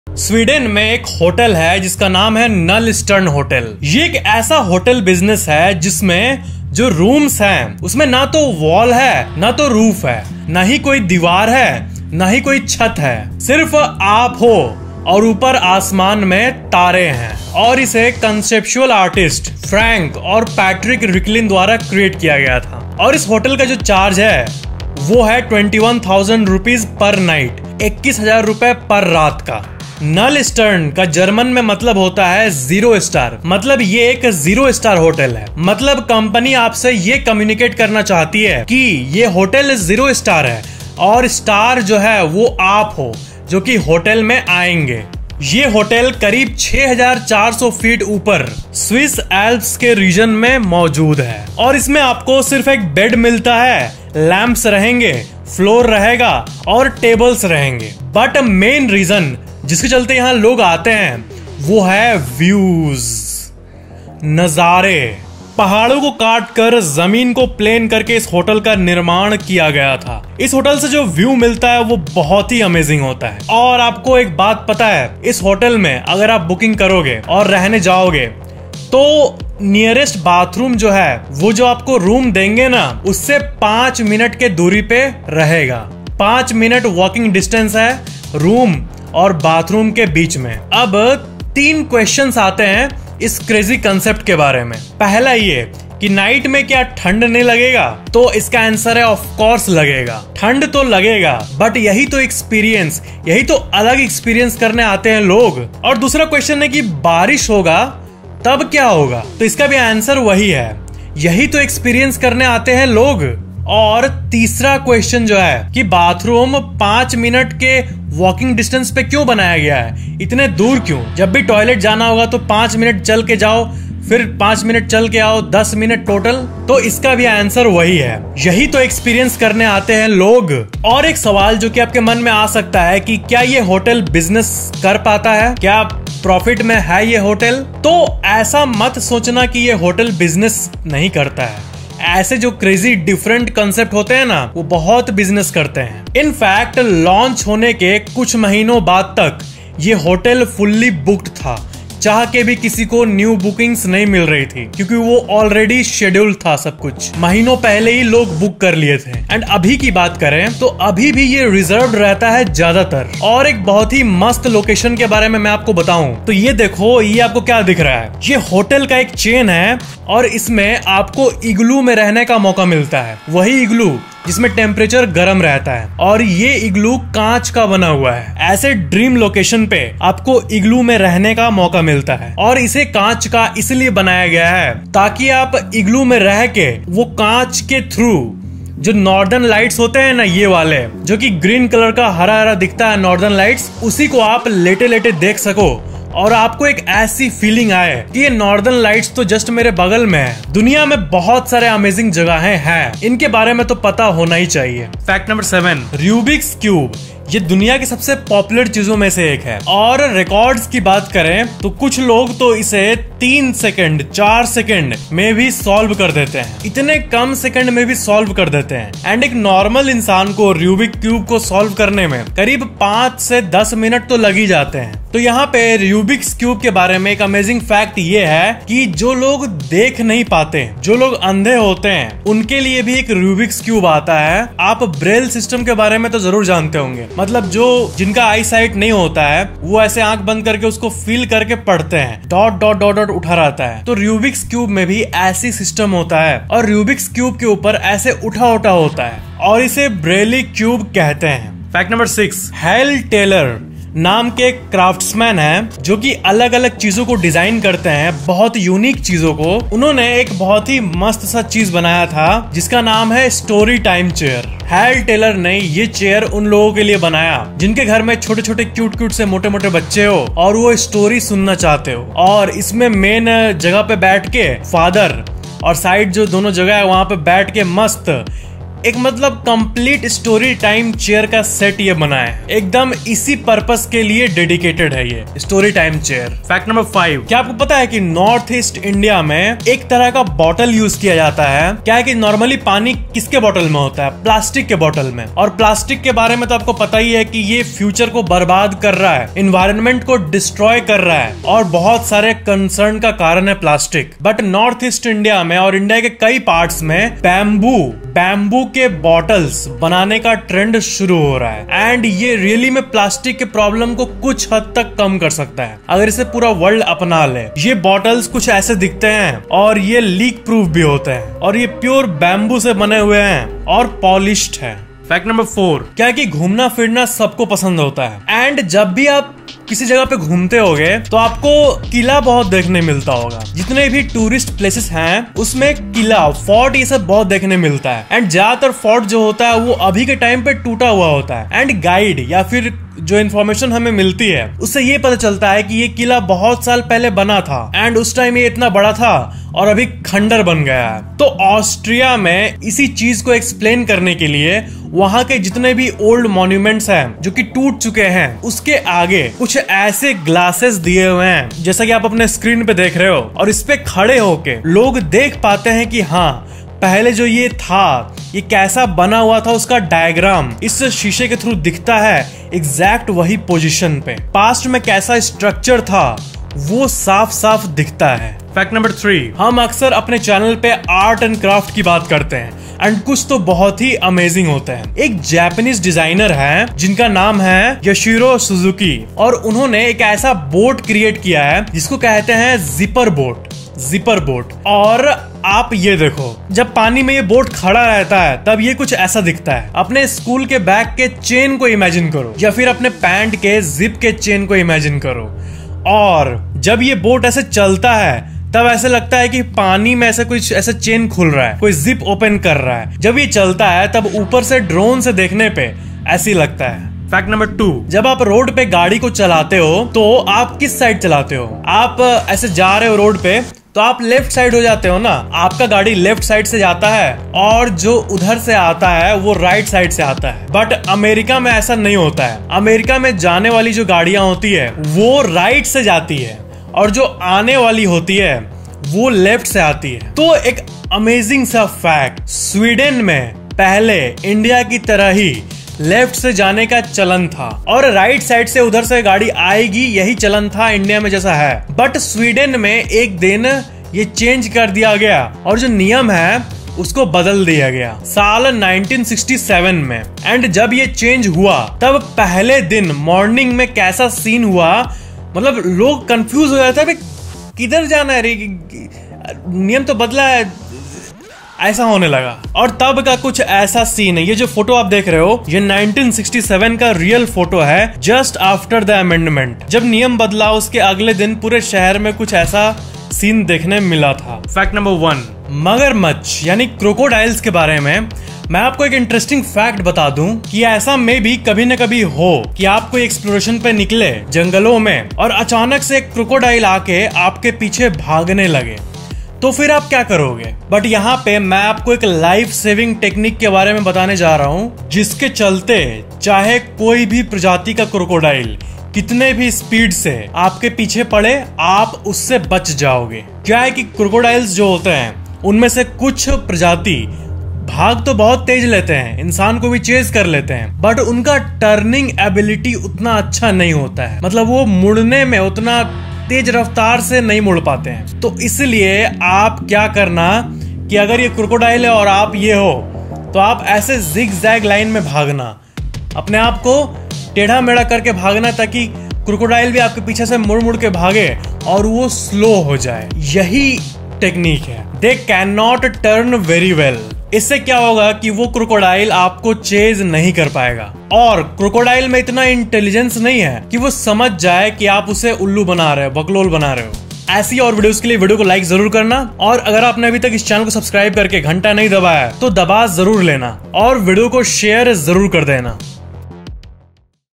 विश्व का पहला 'Open Hotel' - Null Stern Hotel in Sweden & Various Random Facts - TEF Ep 163
Did you know there is a hotel with no walls and roof, for an extraordinary experience, in the alps in sweden? How does a storytime chair looks like?

Let us explore some amazing random facts in this episode 163 of TEF (Top Enigmatic Facts).